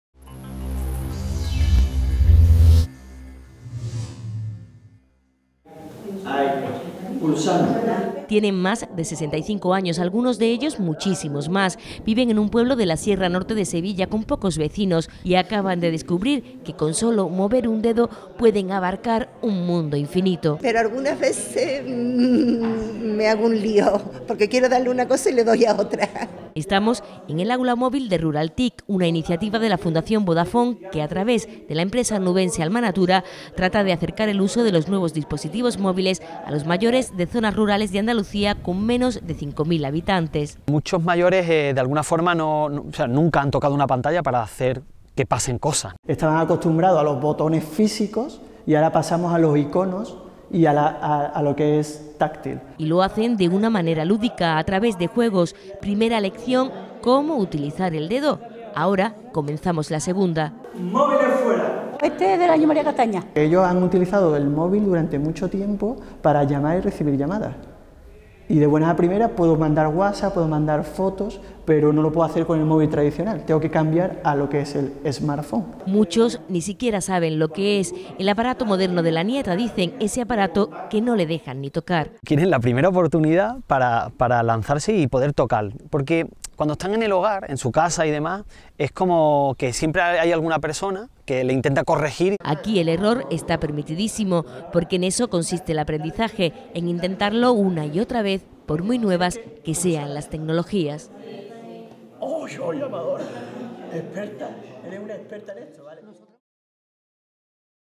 HDL RuralTIC: proyecto para embarcar a los mayores en una relación sin complejos con la tecnología
RuralTIC es una iniciativa de la Fundación Vodafone que,  a través de la empresa onubense Alma Natura, trata de acercar el uso de los nuevos dispositivos móviles a los mayores de las zonas rurales de Andalucía con menos de 5.000 habitantes. Algunos de estos mayores jamás habían visto o usado un smartphone o una tablet.